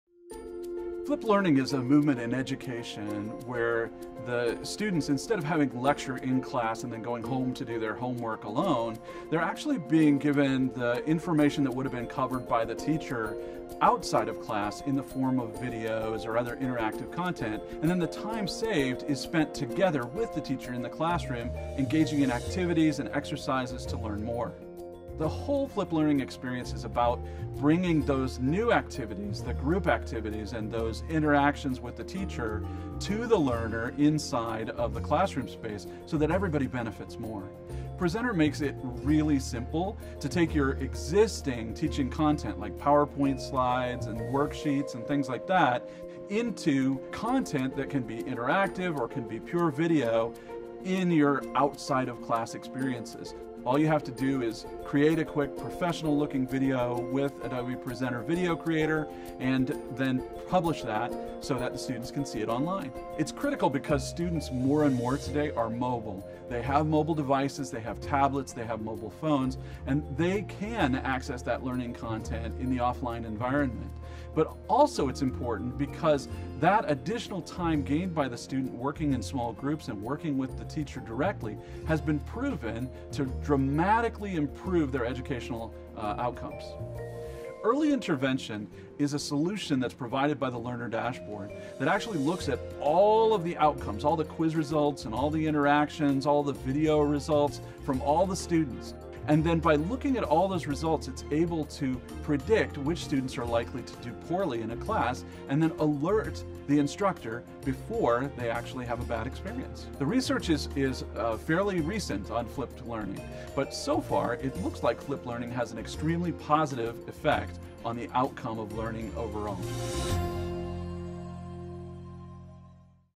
Enable Flipped Learning with Adobe Presenter 10.
Now record yourself and your screen simultaneously to create video lectures for Flipped Learning. Add quizzes, interaction and out-of-the-box assets to make your courses engaging. Track learner performance using built-in analytics.  All of this, with the all-new Adobe Presenter 10.